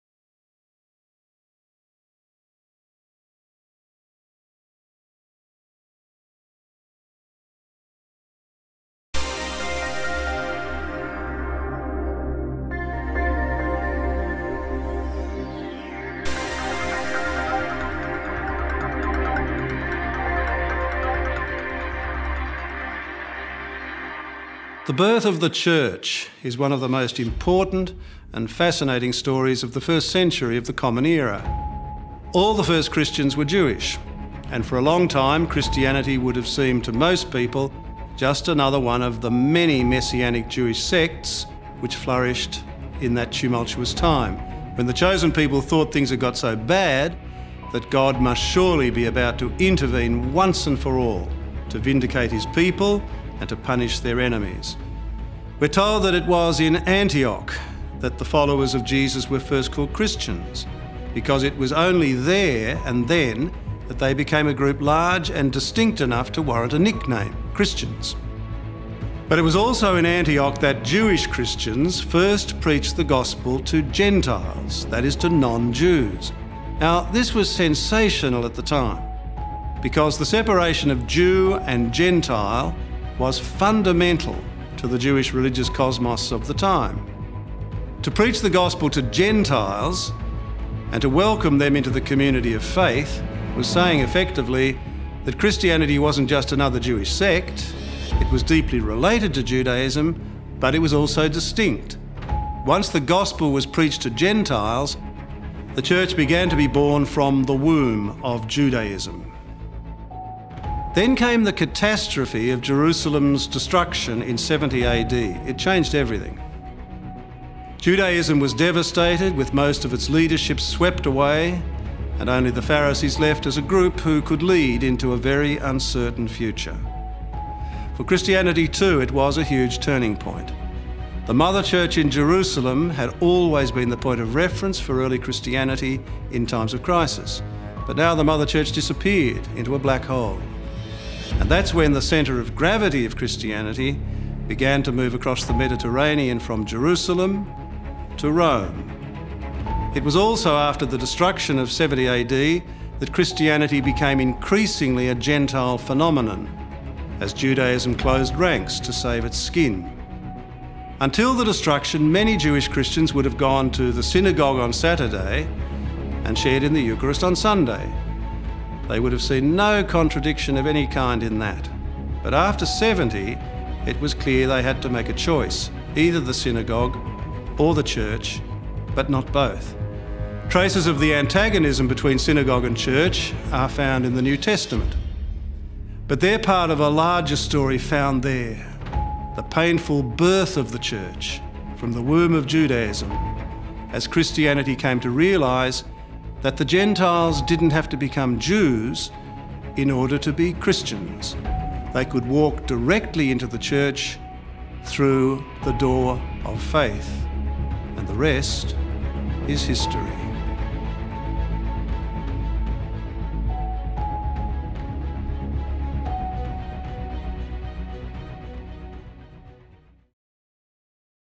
Archbishop Mark Coleridge on the Early Church
Archbishop of Brisbane, Mark Coleridge, discusses the early Church and its birth from the foundation of Judaism. Transcript